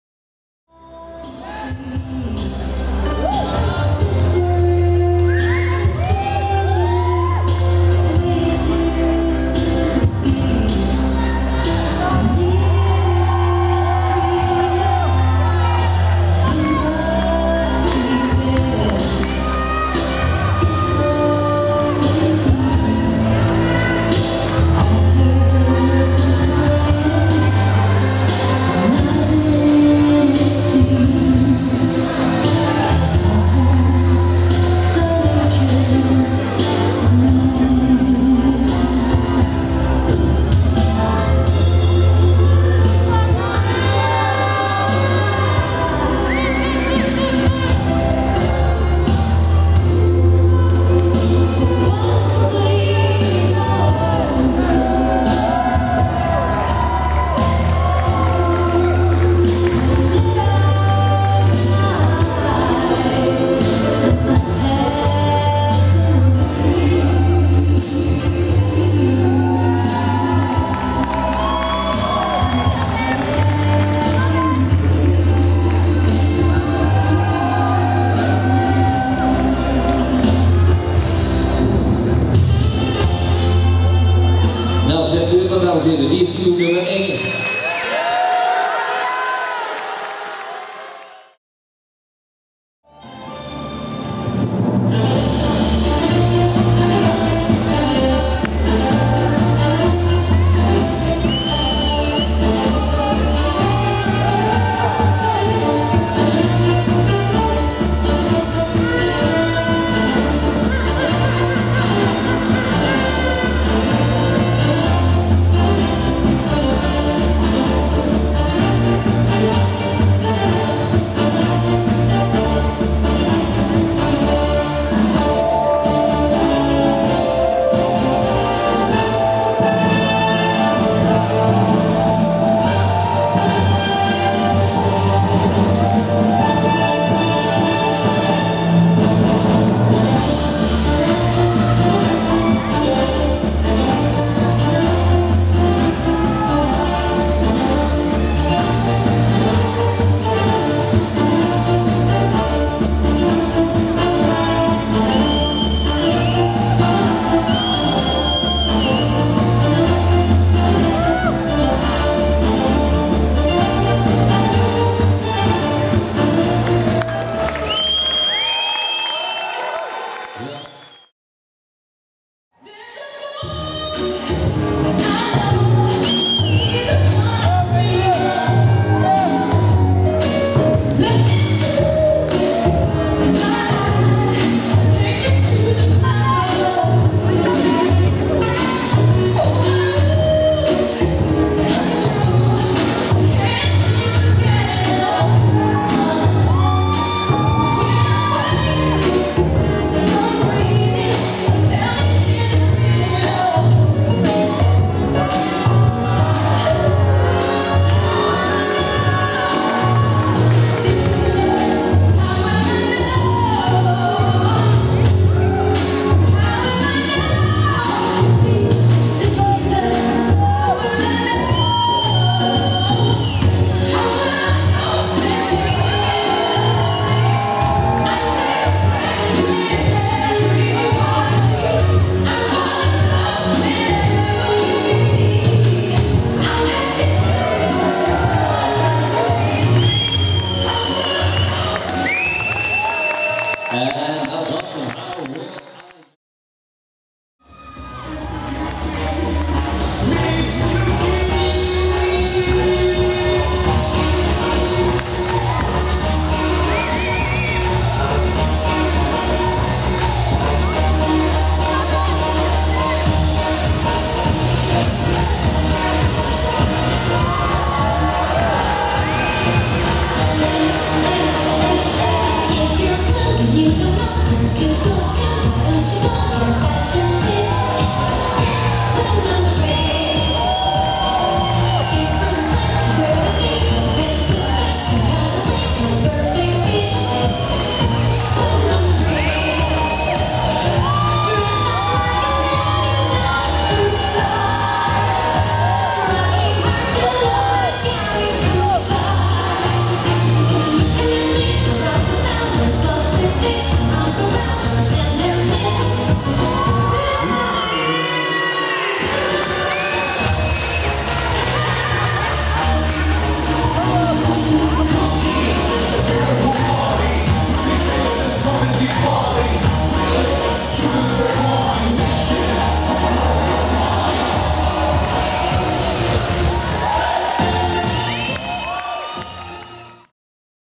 NADB NK Std 2015 Almere Adults B Final
1. 227 Bas Diphoorn / Denise Dols                                 Netherlands
2. 239 Fabio Pecoraro / Nynke van Erven                         Netherlands
3. 230 Reynier Burnaby Lautier / Melline Schilham Netherlands
4. 240 Bert Groot / Laura Swart                                         Netherlands
5. 238 Lieuwe van der Werff / Mirjam Boelens         Netherlands
6. 243 Timo Wolthof / Talitha Pluymaeckers                 Netherlands